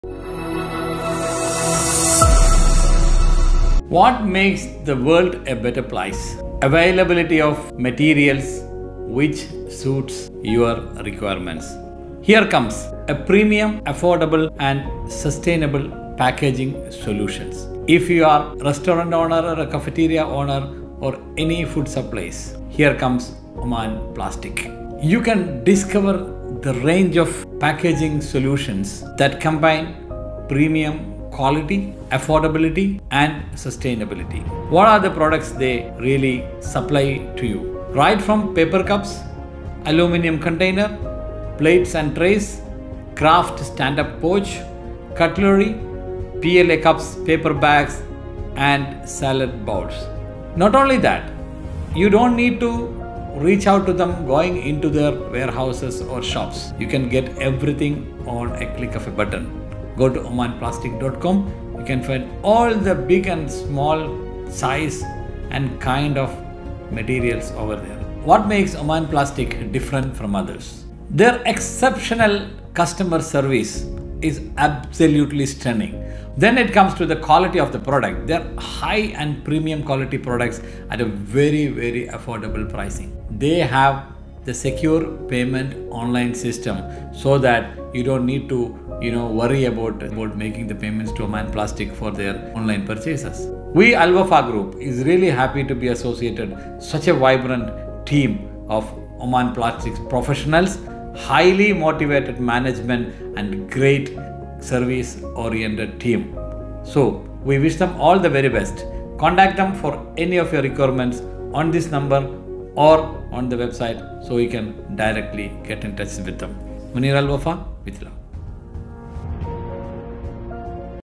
🌟 Episode 33: Oman Plastic LLC | 2 Minute Series with Alwafaa Group 🌟
We are excited to present Episode 33 of our 2 Minute Series!

In this episode, we shine the spotlight on the innovative Oman Plastic LLC. Oman Plastic LLC is a leader in providing premium, affordable, and sustainable packaging solutions, catering to various industries with precision and excellence.

🔹 Premium Packaging Solutions
🔹 Affordable and Sustainable Options
🔹 Commitment to Quality and Innovation

Explore how Oman Plastic LLC can meet your packaging needs with their wide range of products, including paper cups, aluminium containers, plates & trays, kraft stand-up pouches, cutlery, PLA cups, paper bags, and salad bowls.